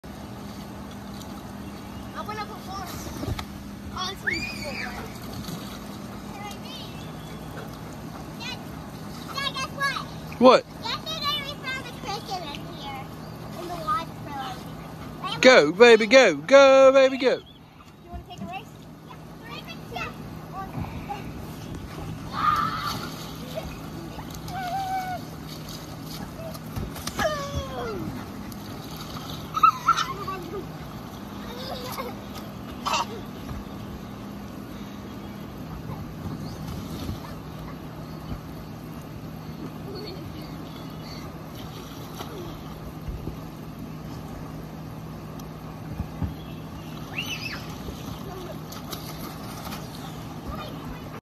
H2O Go from Sam’s Review.
This product is awesome. My only complaint is that the slide end length you slide in to is too short. It needs to be a few feet longer so you don’t slam in to the wall or go over the wall off the slide.